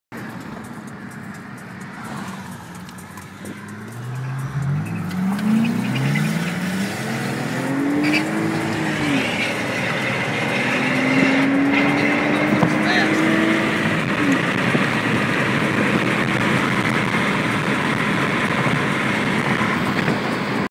Josh BMW racing Corvette
Josh and Justin racing outside Afterlife, August 9, 2009